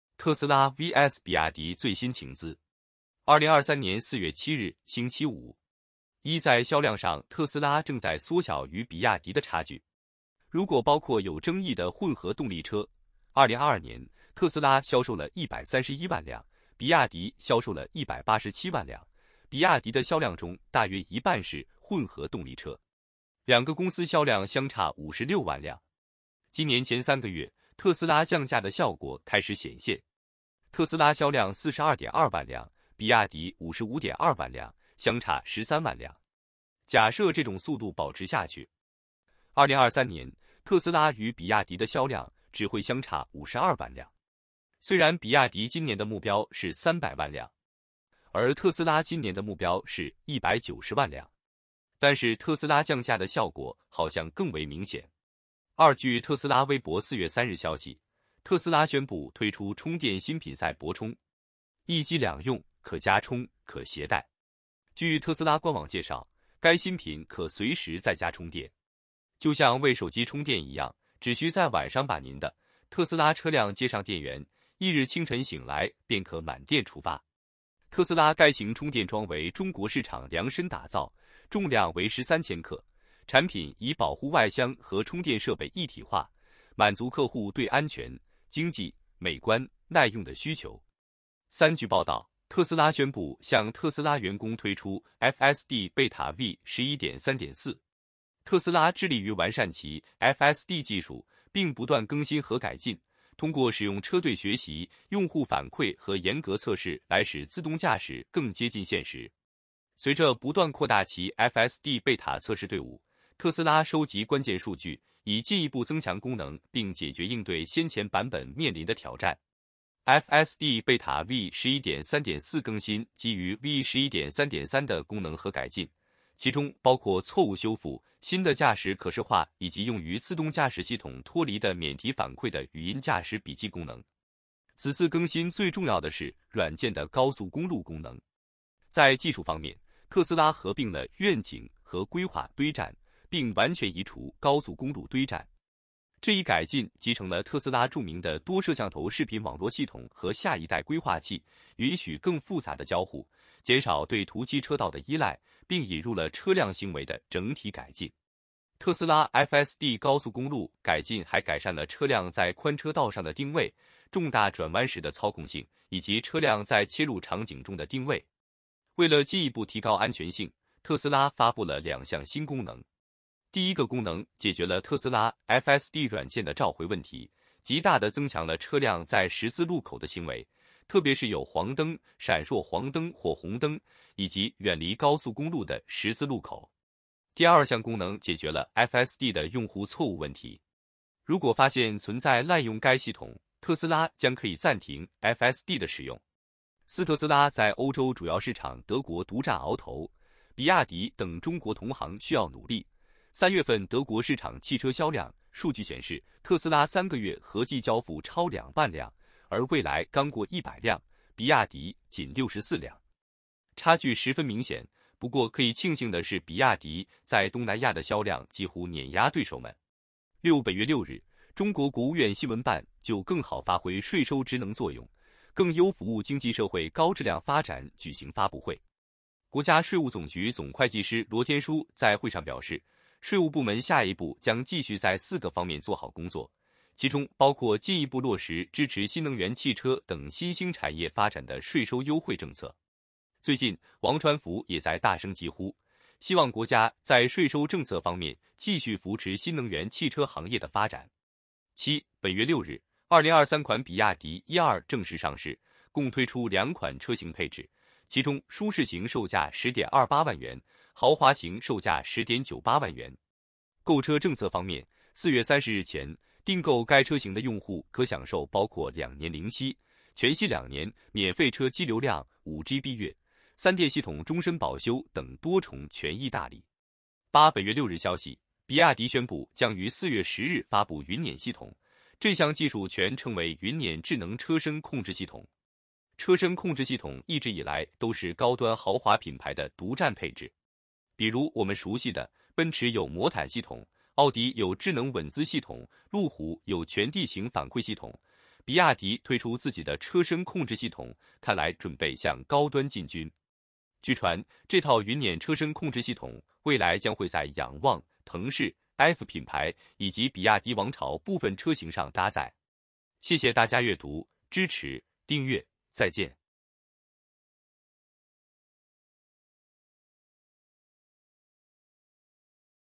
特斯拉VS比亚迪最新情资 2023年04月07日（星期五）
1.在销量上特斯拉正在缩小与比亚迪的差距
如果包括有争议的混合动力车，2022年，特斯拉销售了131万辆，比亚迪销售了187万辆，比亚迪的销量中大约一半是混合动力车。
两个公司销量相差56万辆
今年前三个月，特斯拉降价的效果开始显现。
特斯拉销量42.2万辆，比亚迪55.2万辆，相差13万辆
假设这种速度保持下去，
2023年，特斯拉与比亚迪的销量只会相差52万辆
虽然比亚迪今年的目标是300万辆，
而特斯拉今年的目标是190万辆。但是特斯拉降价的效果好像更为明显。
2.据特斯拉微博4月3日消息，特斯拉宣布推出充电新品赛博充。一机两用，可家充，可携带。

据特斯拉官网介绍，该新品可随时在家充电。就像为手机充电一样，只需在晚上把您的 Te­s­la 车辆接上电源，翌日清晨醒来便可满电出发。

特斯拉该型充电桩为中国市场量身打造，重量为13千克，产品以保护外箱和充电设备一体化，满足客户对安全，经济，美观，耐用的需求。
3.据报道，特斯拉宣布向特斯拉员工推出FSD Be­ta v11.3.4。
特斯拉致力于完善其FSD技术，并不断更新和改进，通过使用车队学习、用户反馈和严格测试来使自动驾驶更接近现实。随着不断扩大其FSD Be­ta测试队伍，特斯拉收集关键数据，以进一步增强功能并解决应对先前版本面临的挑战。
FSD Be­ta v11.3.4更新基于v11.3.3的功能和改进，其中包括错误修复、新的驾驶可视化以及用于Au­t­o­p­i­l­ot脱离的免提反馈的语音驾驶笔记功能。
此次更新最重要的是软件的高速公路功能。在技术方面，特斯拉合并了“愿景”和“规划”堆栈，并完全移除高速公路堆栈。这一改进集成了特斯拉著名的多摄像头视频网络系统和下一代规划器，允许更复杂的交互，减少对涂漆车道的依赖，并引入了车辆行为的整体改进。

特斯拉FSD高速公路改进还改善了车辆在宽车道上的定位，重大转弯时的操控性，以及车辆在切入场景中的定位。

为了进一步提高安全性，特斯拉发布了两项新功能。第一个功能解决了特斯拉FSD软件的召回问题，极大地增强了车辆在十字路口的行为，特别是有黄灯、闪烁黄灯或红灯，以及远离高速公路的十字路口。第二项功能解决了FSD的用户错误问题。如果发现存在滥用该系统，特斯拉将可以暂停FSD的使用。
4.特斯拉在欧洲主要市场德国独占鳌头，比亚迪等中国同行需要努力，3月份德国市场汽车销量数据显示：特斯拉3个月合计交付超2万辆，而蔚来刚过100辆，比亚迪仅64辆。差距十分明显，不过可以庆幸的是比亚迪在东南亚的销量几乎碾压对手们。
6.本月6日，中国国务院新闻办就“更好发挥税收职能作用 更优服务经济社会高质量发展”举行发布会。国家税务总局总会计师罗天舒在会上表示，税务部门下一步将继续在四个方面做好工作，其中包括进一步落实支持新能源汽车等新兴产业发展的税收优惠政策。最近王传福也在大声疾呼，希望国家在税收政策方面继续扶持新能源汽车行业的发展。
7.本月6日，2023款比亚迪 e2 正式上市，共推出两款车型配置，其中舒适型售价 10.28 万元，豪华型售价 10.98 万元。购车政策方面，4月30日前，订购该车型的用户可享受包括2年0息，全系2年免费车机流量（5GB/月），三电系统终身保修等多重权益大礼。
8.本月6日消息，比亚迪宣布将于4月10日发布云辇系统，这项技术全称为“云辇智能车身控制系统”。
车身控制系统一直以来都是高端豪华品牌的独占配置。比如我们熟悉的，奔驰有魔毯系统，奥迪有智能稳姿系统、路虎有全地形反馈系统，比亚迪推出自己的车身控制系统，看来准备向高端进军。
据传，这套云辇车身控制系统未来将会在仰望、腾势、F 品牌以及比亚迪王朝部分车型上搭载。